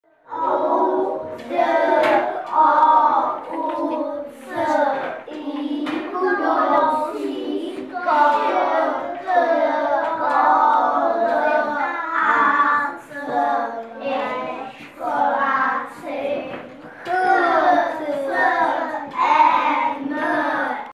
Předškoláci MŠ MiniSvět v opravdové škole
Předškoláci mateřské školky MiniSvět dnes měli možnost navštívit ZŠ v Senohrabech. Děti první třídy pro nás připravily zajímavý program a páťáci nám ukázali celou školu. Bylo to prima!